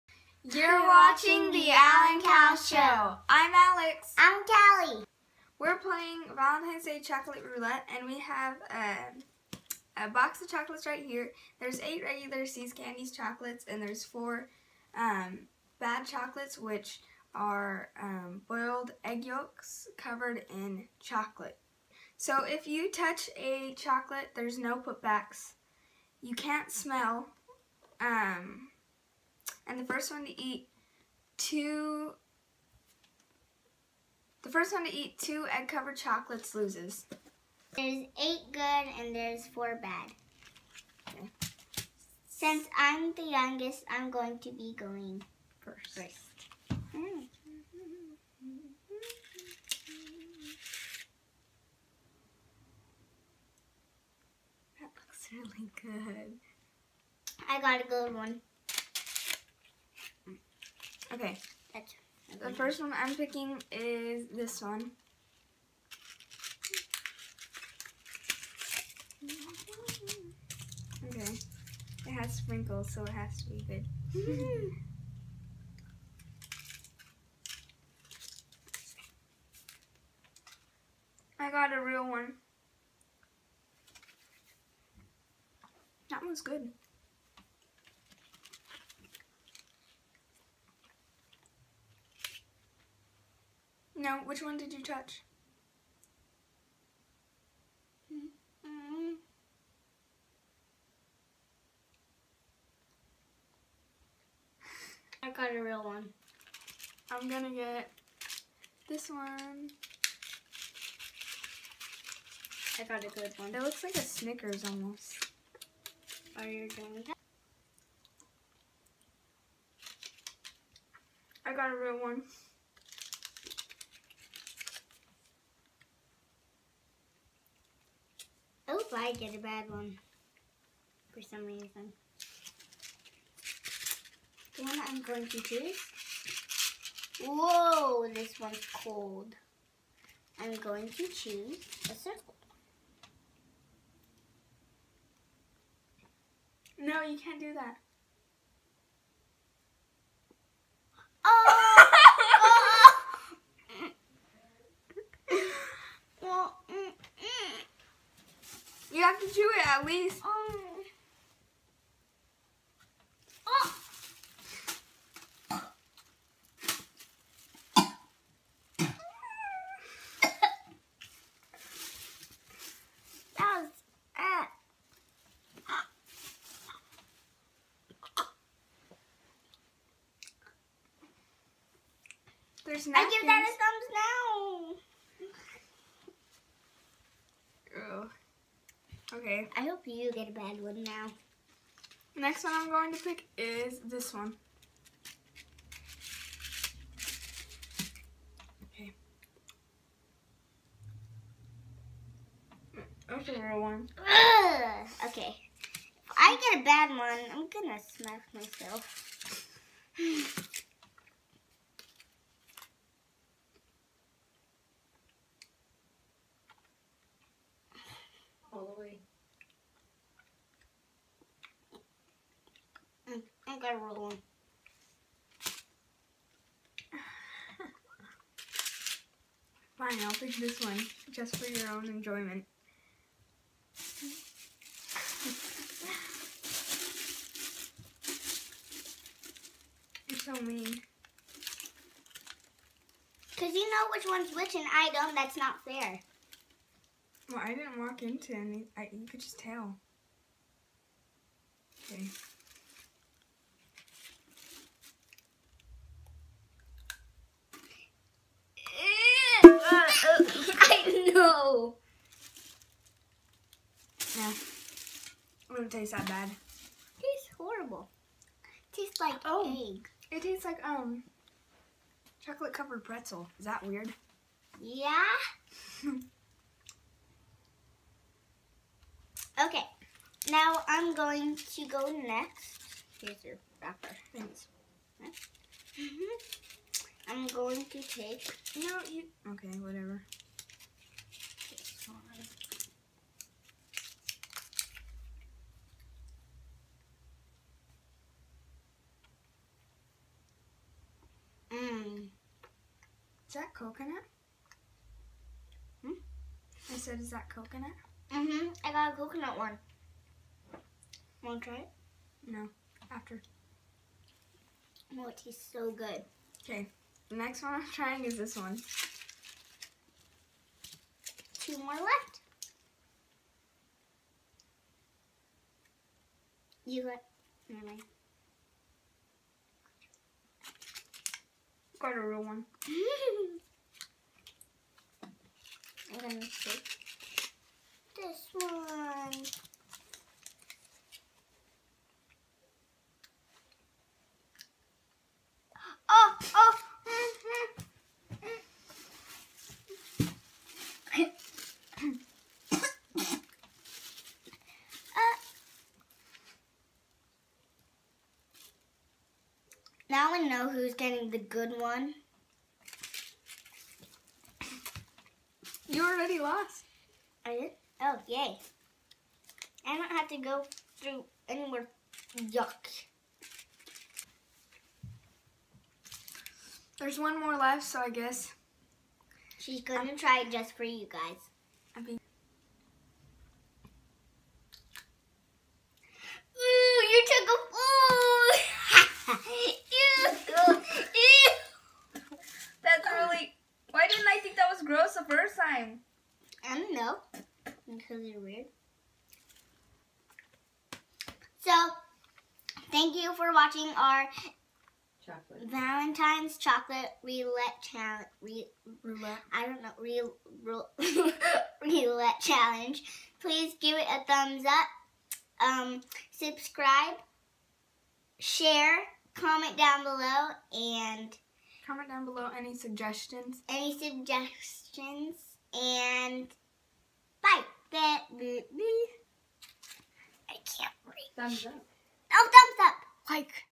Valentine's Chocolate Roulette!
Happy Valentine's Day Everyone!!! We hope you liked our video. We had fun! Let us know if you try it! We hope you have an amazing Valentine's Day!

Products we used:
See's Candy 8oz. Classic Red Heart

SEND US LETTERS!
United States